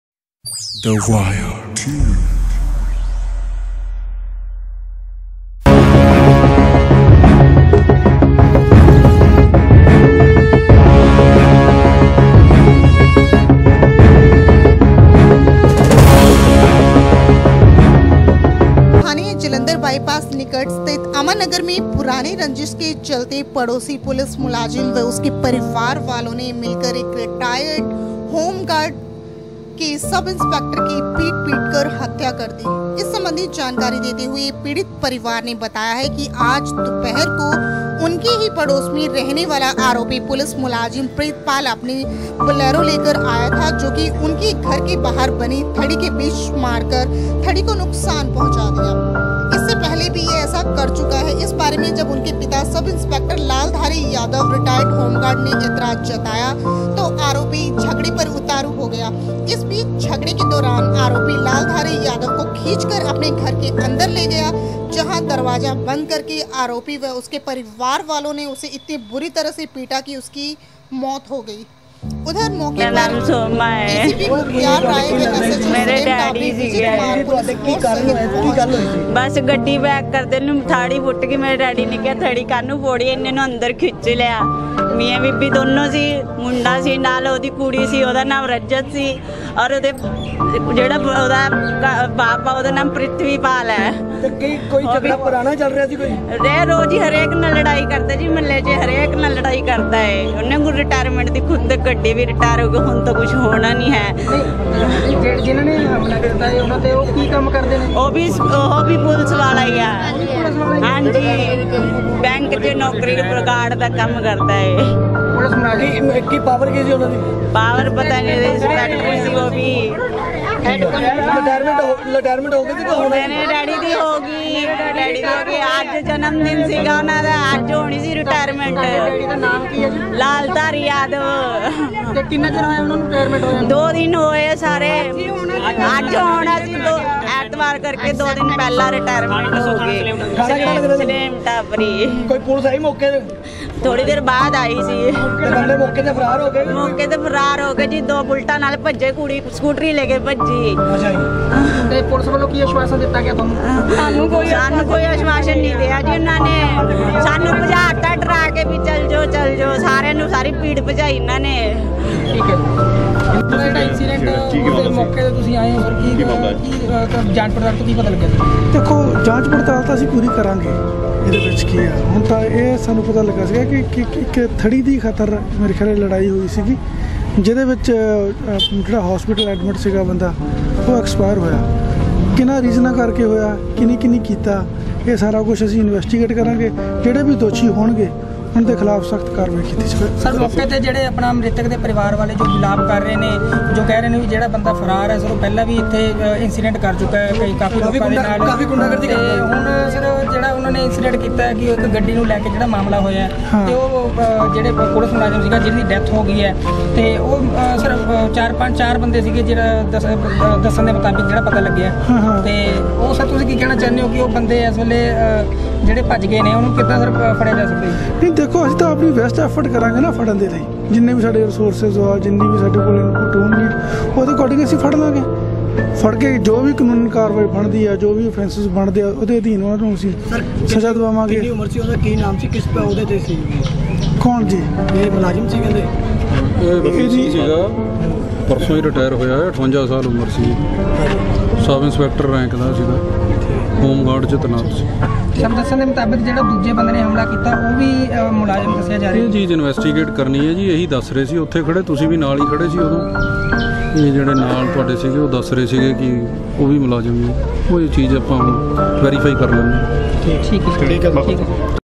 Thewiretv||Police officer killed by police||Subscribe channel
The wire tv is the news channel in world.
The wire tv is the best channel लुधियाना के स्थानीय जालंधर बाइपास निकट स्थित अमन नगर में उस समय सनसनी फैल गई जब पुरानी रंजिश के चलते पड़ोसी सर्विस मुक्त पुलिस मुलाजिम व उसके परिवार वालों ने मिलकर एक रिटायर्ड होमगार्ड के सब-इंस्पेक्टर की पीट-पीट कर हत्या कर दी।